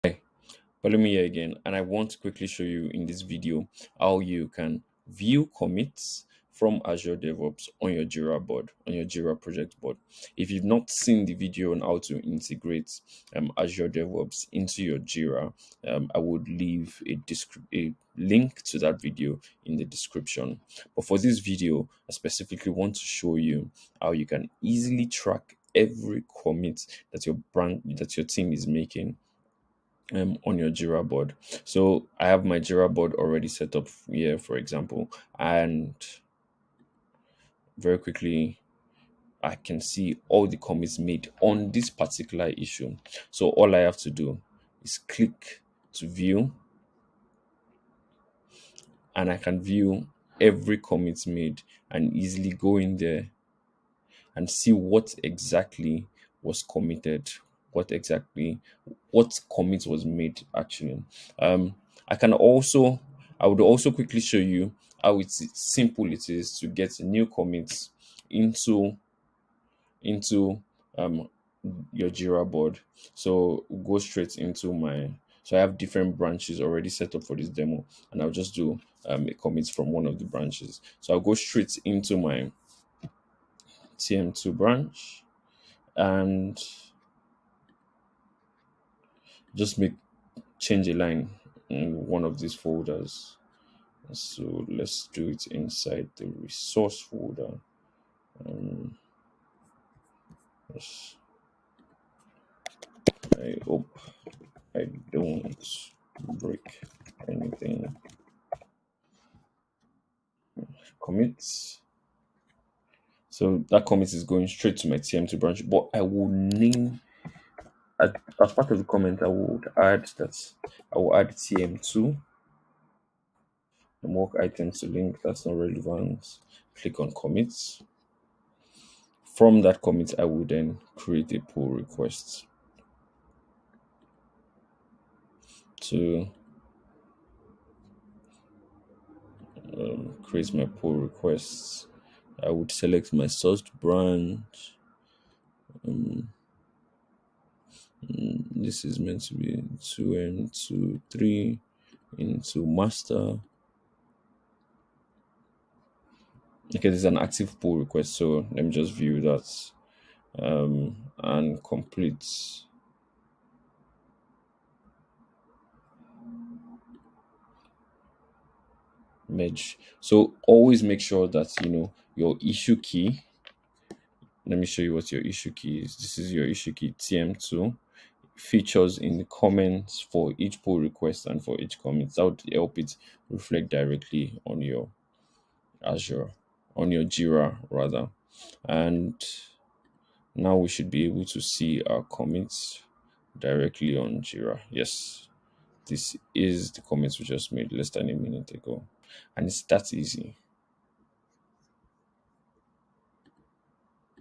How to VIEW COMMITS from Azure DevOps in JIRA
Learn how to view commits from Azure DevOps in Jira.